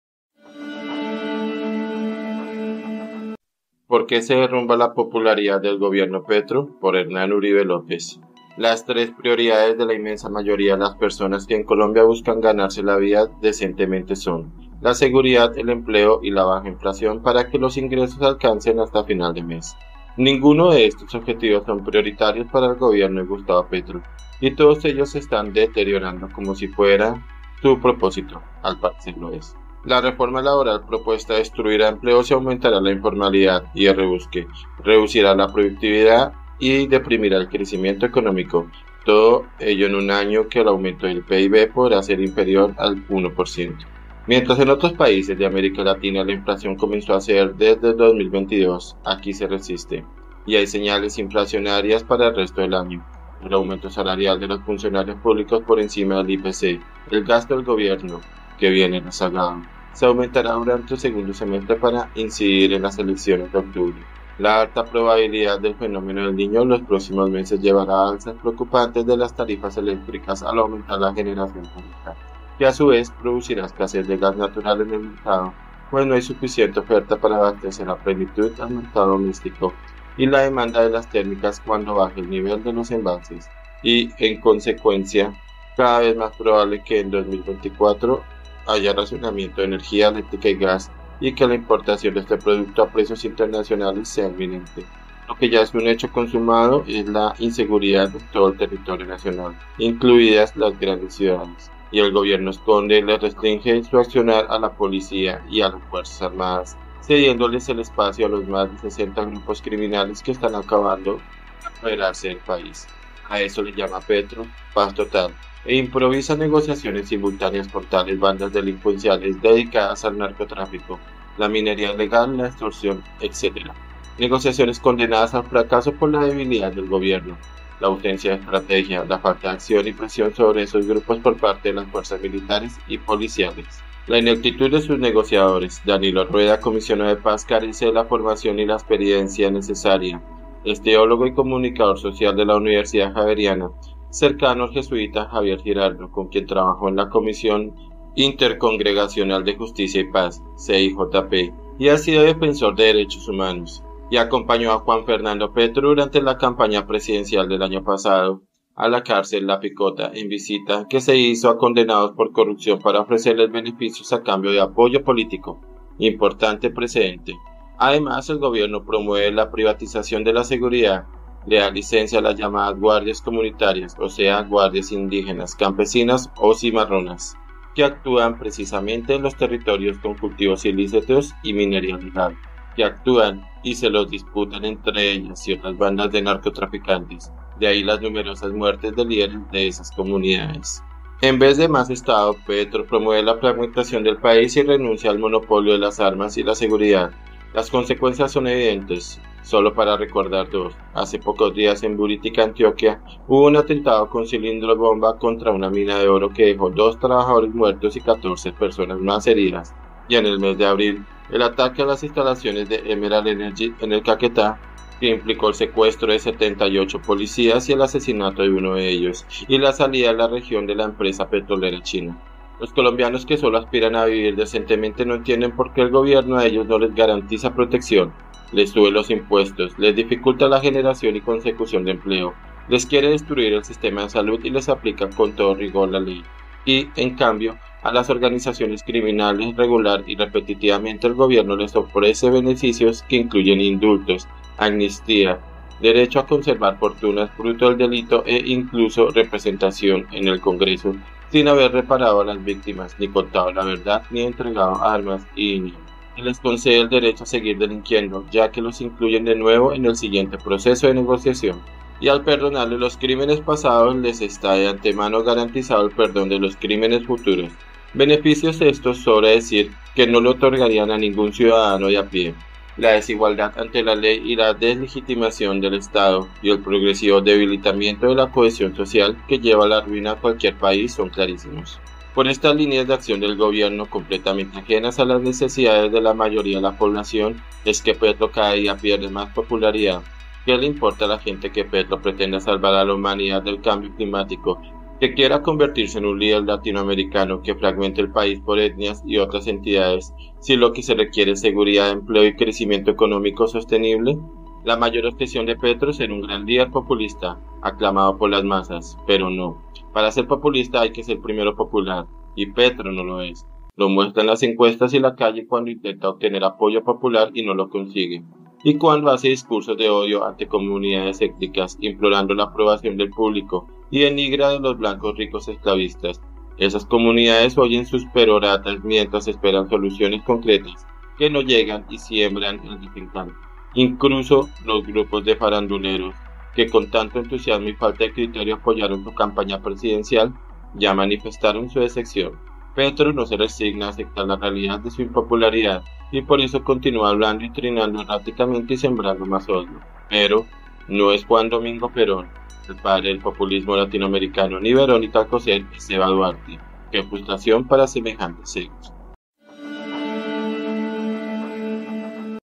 POR QUÉ SE DERRUMBA LA POPULARIDAD DEL GOBIERNO PETRO 🔴 Por Hernán Uribe López
Las tres prioridades de la inmensa mayoría de las personas que en Colombia buscan ganarse la vida decentemente son: la seguridad, el empleo y la baja inflación para que los ingresos alcancen hasta final de mes. Ninguno de estos objetivos son prioritarios para el gobierno de Gustavo Petro. Y todos ellos se están deteriorando, como si ese fuera su propósito. Al parecer lo es.
columna Hernán Uribe López